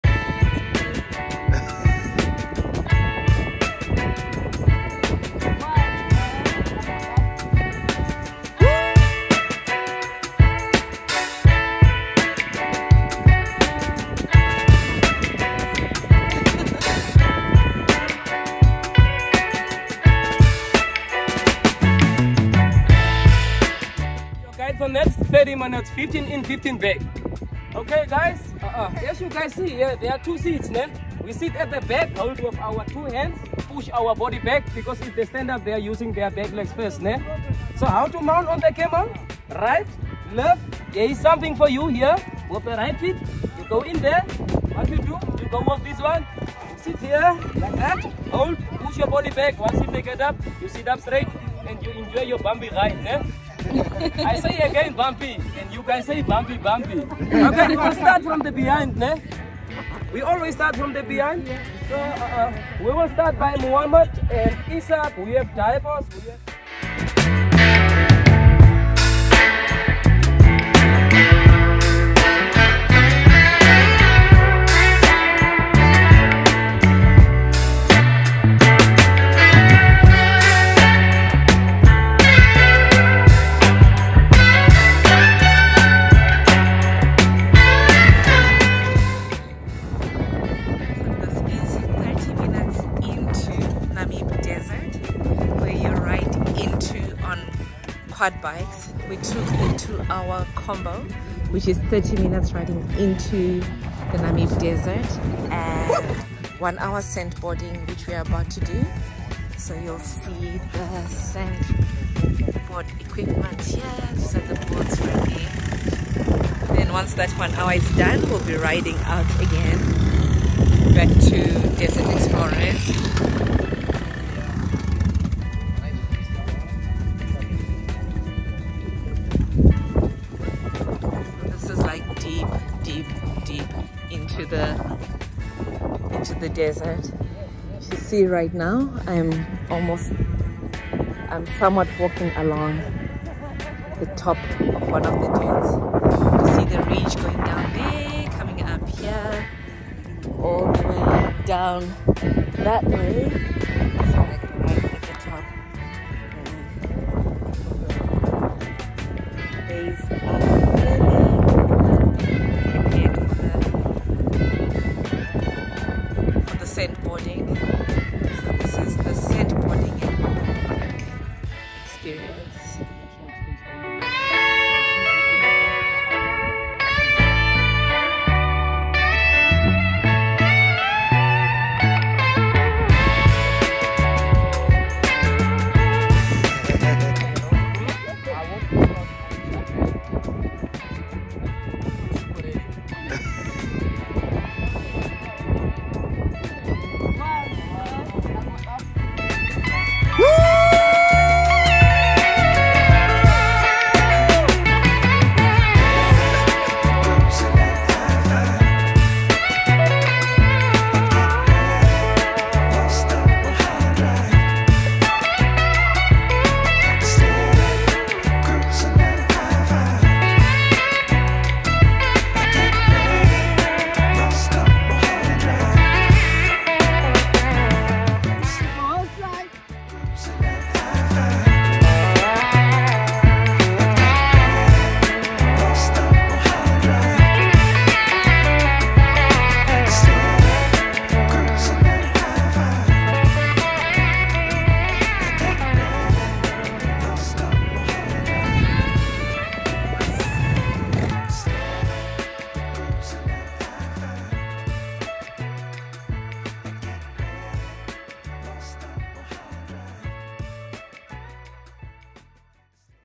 Surf the Namibian Dunes
Our latest adventure in Swakopmund Namibia was really awesome.